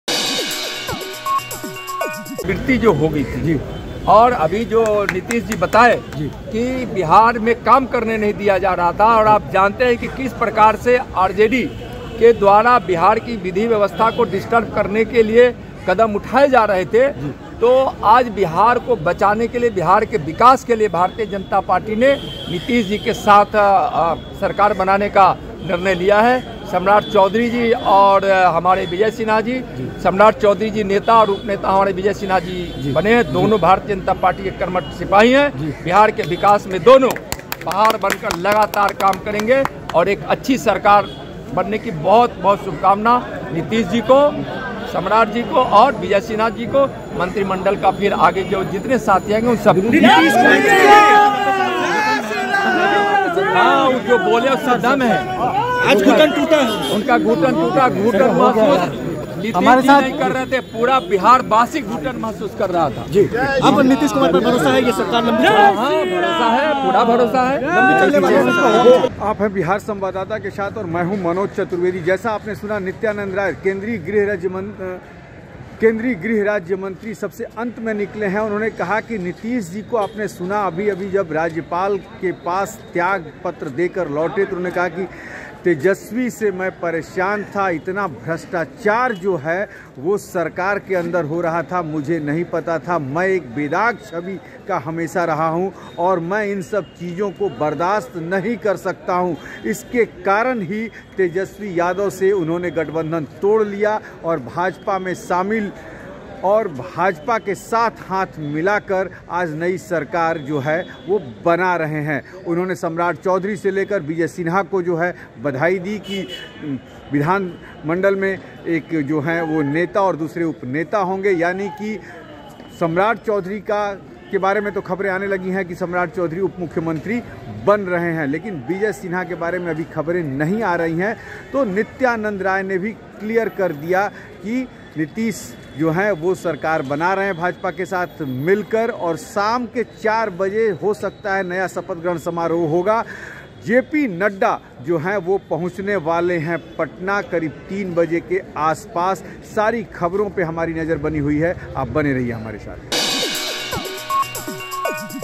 तेजस्वी के भ्र्ष्टाचार से ऊब चुके थे नीतीश ,हमनें बिहार को बचाने के लिए समर्थन दिया-नित्यानंद
..Nitish was fed up with Tejashwi's corruption
We supported to save Bihar – Nityanand

bihar news
bihar news today
patna news